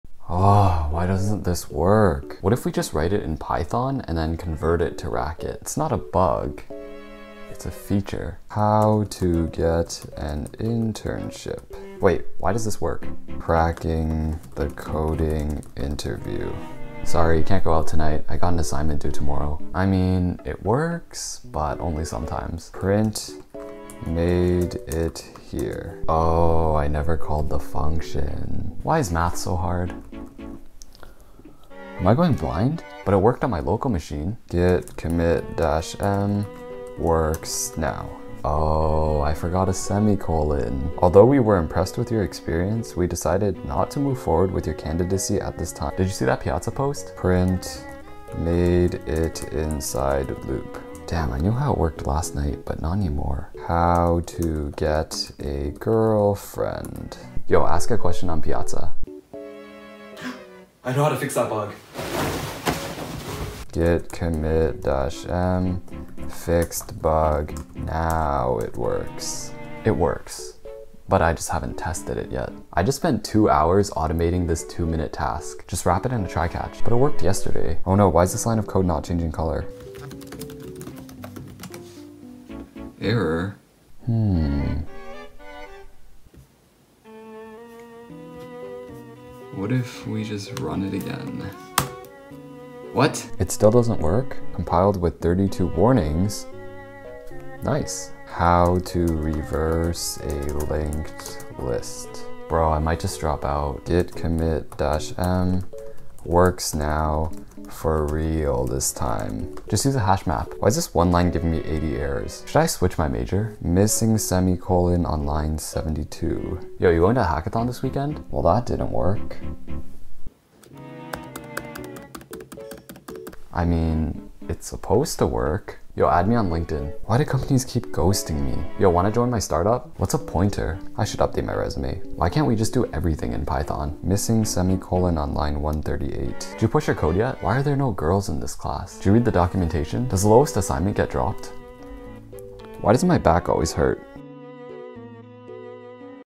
Computer Science Majors Be Like
Being a Computer Science student is hard. From buggy code, to internship struggles, here's what it's like. Might make a part 2 if this video does well 👀 👀

Comment the most accurate/relatable phrase below!

This video was inspired by Ali Malik's "Stuff Computer Science Students Say PARODY" Check it out